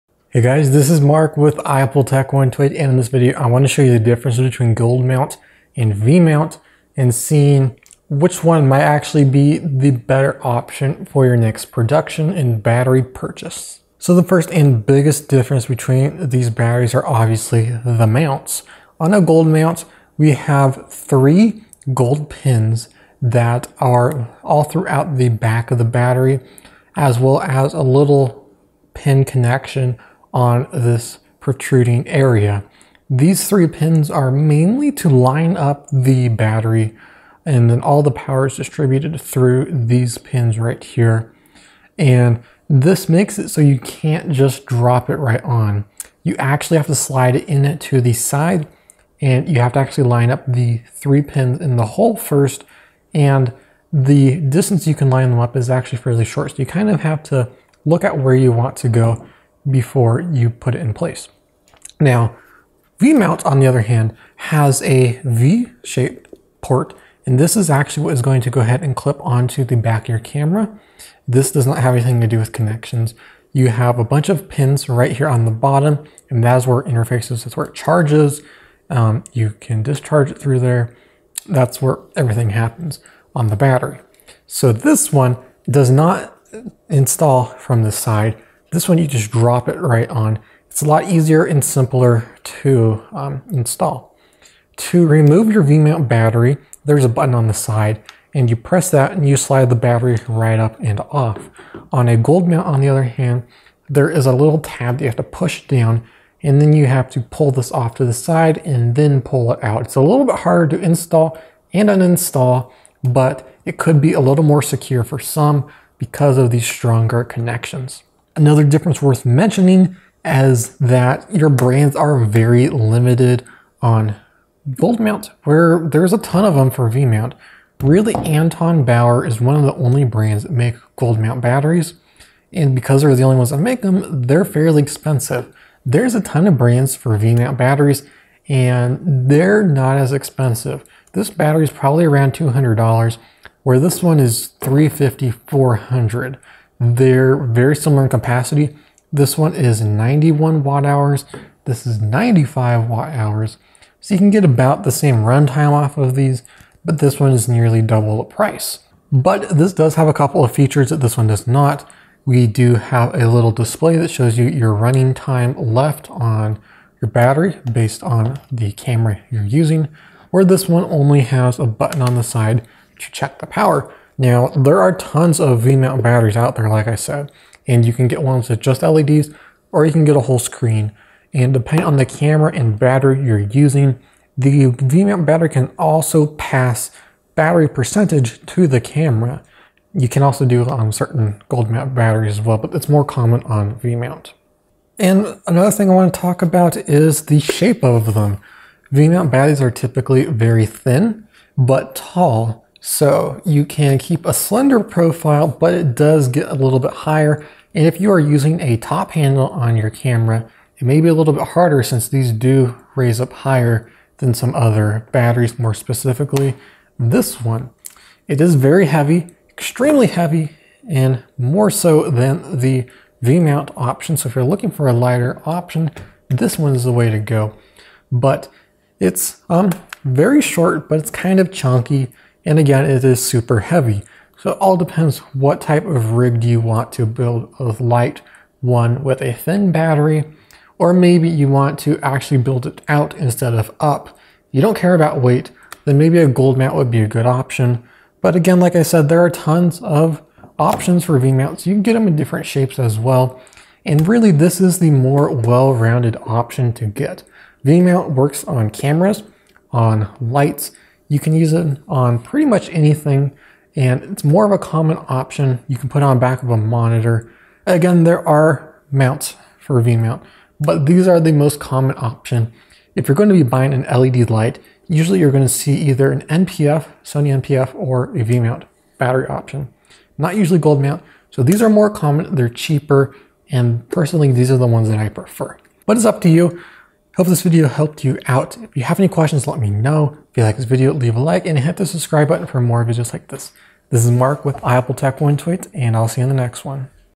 Gold Mount Vs. V-Mount
In this video we explain the differences between gold mount and v-mount batteries. They are used for many different things and have features that differer.